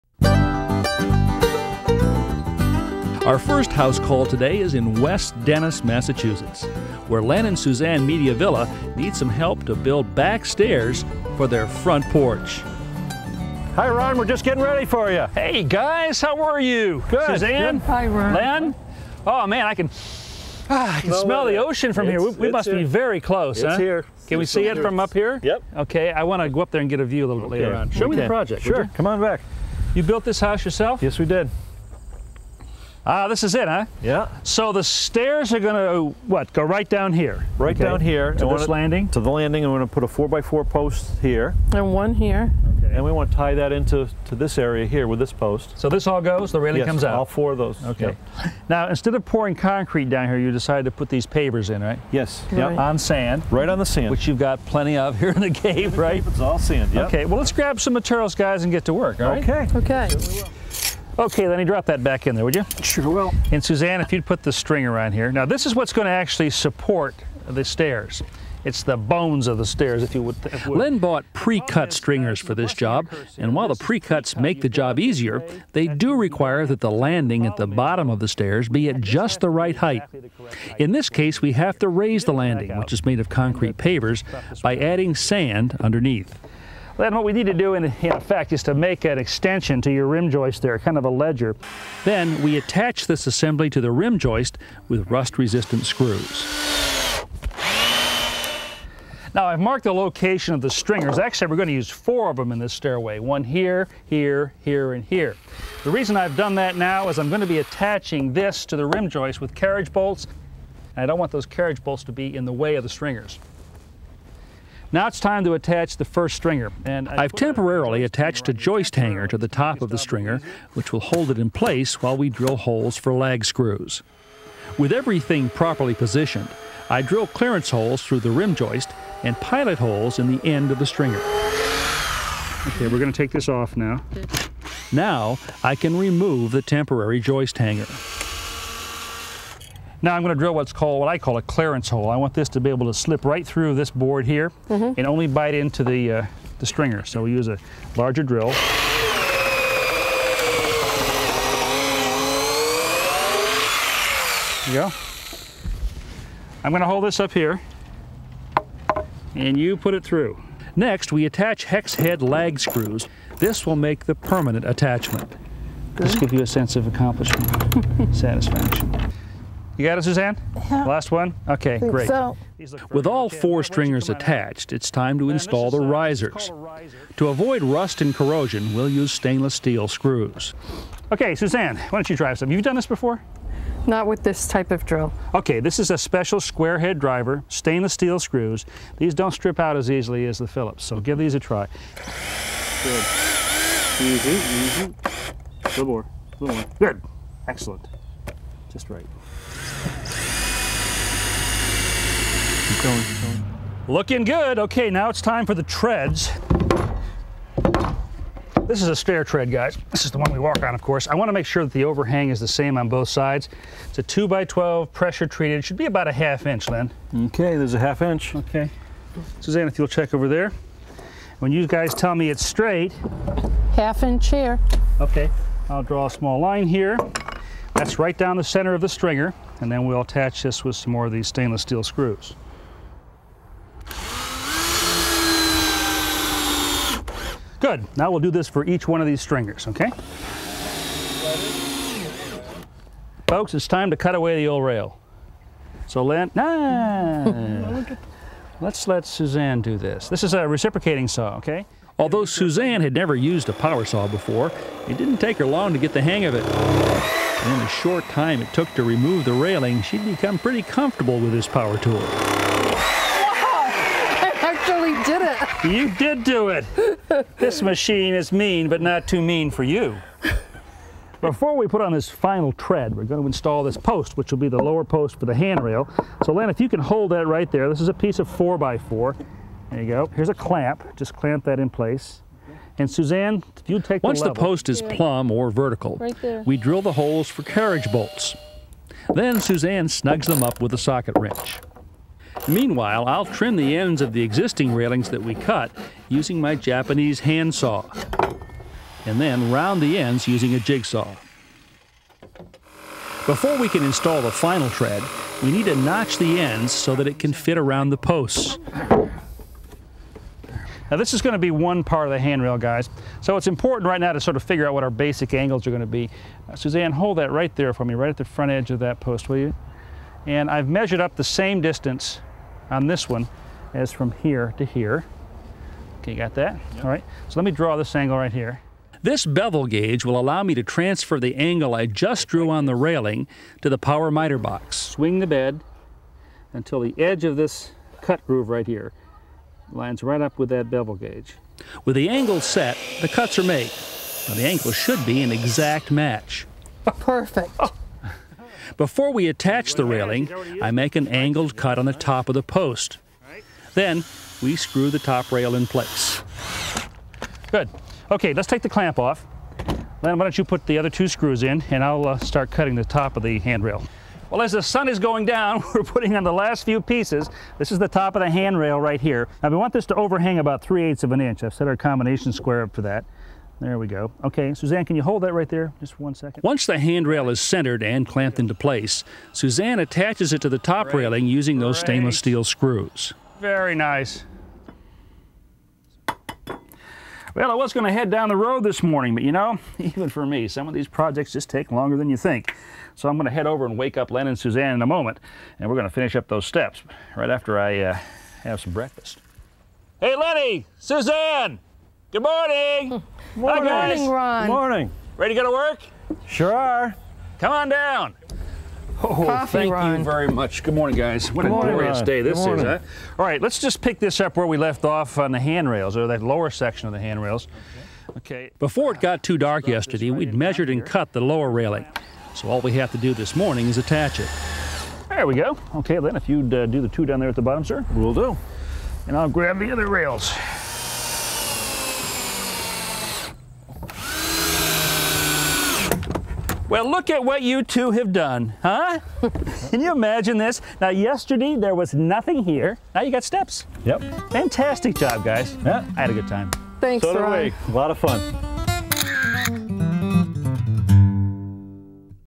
How to Build a Porch Stairway
Watch this project now to see how builds a porch stairway with pre-cut Stringers.